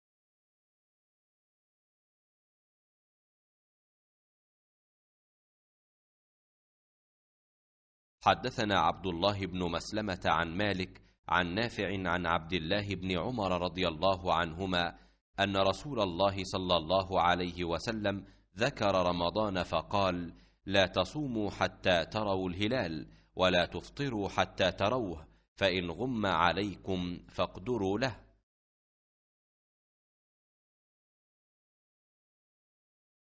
صحيح البخارى - كتاب الصوم - 1906 - باب قول النبي صلى الله عليه وسلم: «إذا رأيتم الهلال فصوموا وإذ...
صحيح البخارى - كتاب الصوم - 1906 - باب قول النبي صلى الله عليه وسلم: «إذا رأيتم الهلال فصوموا وإذا رأيتموه فأفطروا»
حدثنا عبد الله بن مسلمة، عن مالك، عن نافع، عن عبد الله بن عمر ـ رضى الله عنهما ـ أن رسول الله صلى الله عليه وسلم ذكر رمضان فقال ‏  "‏ لا تصوموا حتى تروا الهلال، ولا تفطروا حتى تروه، فإن غم عليكم فاقدروا له ‏"  ‏‏.‏